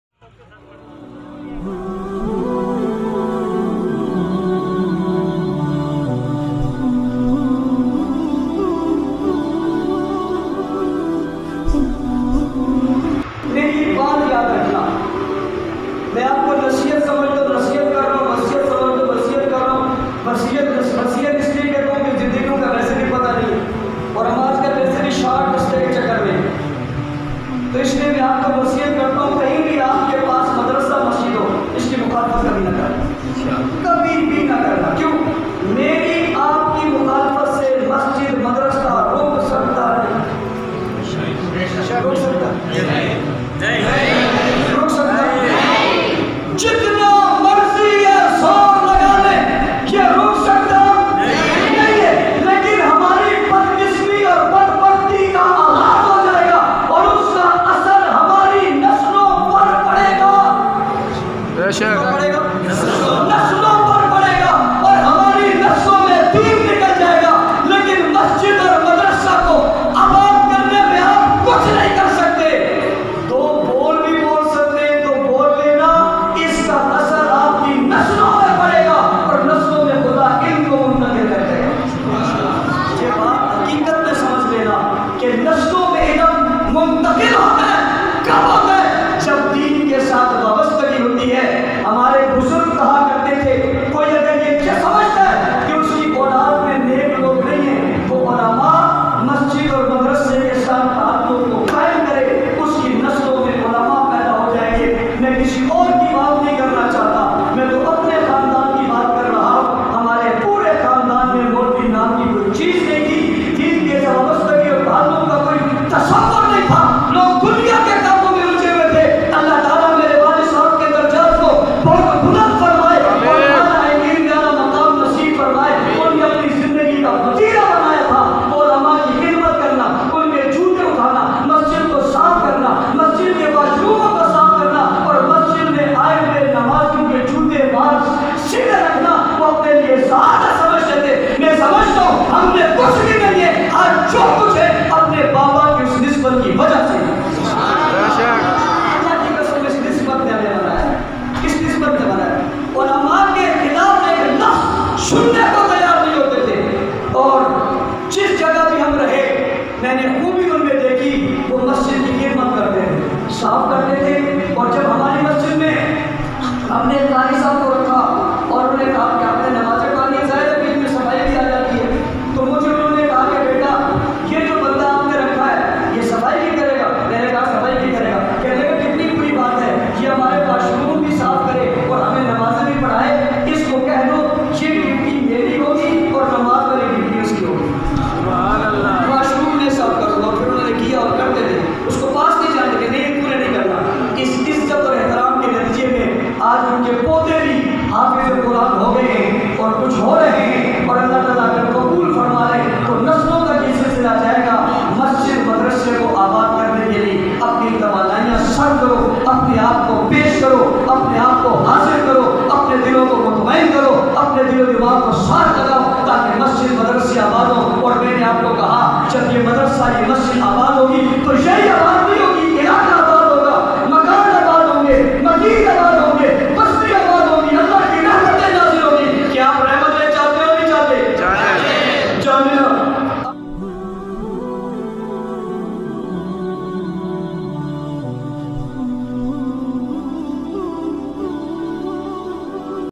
Landhi||Allama Aurangzaib Farooqui Shb ki Nasihat Aur Wassiat||Bayan Lala Abad
Allama Aurangzaib Farooqui Bayan 18Nov2020 Durs e Quran Landhi Lala Abad Madarsa Abu Zar Ghaffari Rz
Mozoh Karkuno k Naam Eham Pegam